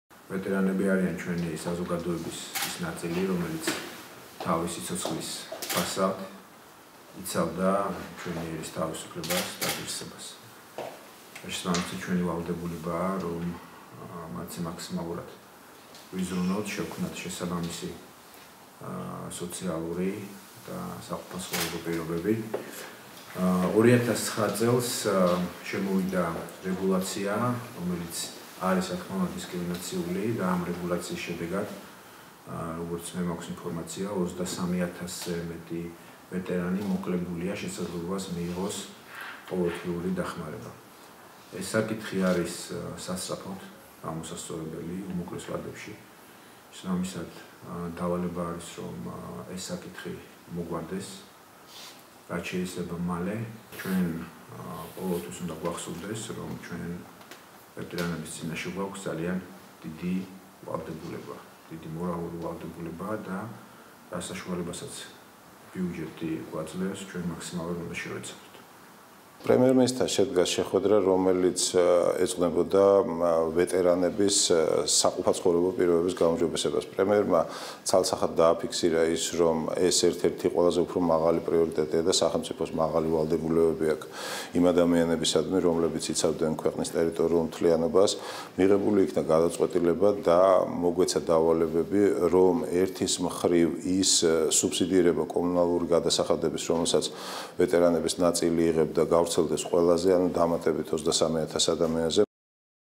მამუკა ბახტაძე - ვეტერანებს ღირსეულისოციალური პირობები უნდა შევუქმნათ
ვეტერანები ჩვენი საზოგადოების ის ნაწილია, რომელიც თავისი სიცოცხლის ფასად იცავდა ჩვენი ერის თავისუფლებას და ღირსებას, შესაბამისად, ჩვენი ვალდებულებაა, რომ მათზე მაქსიმალურად ვიზრუნოთ და შევქმნათ შესაბამისი სოციალური და საყოფაცხოვრებო პირობები", - ამის შესახებ საქართველოს პრემიერ-მინისტრმა მამუკა ბახტაძემ საქართველოს ოკუპირებული ტერიტორიებიდან დევნილთა, შრომის, ჯანმრთელობისა და სოციალური დაცვის მინისტრ დავით სერგეენკოსთან, ფინანსთა მინისტრ ივანე მაჭავარიანთან და ვეტერანების საქმეთა სახელმწიფო სამსახურის დირექტორთან, ვლადიმერ იმნაძესთან შეხვედრის შემდეგ განაცხადა.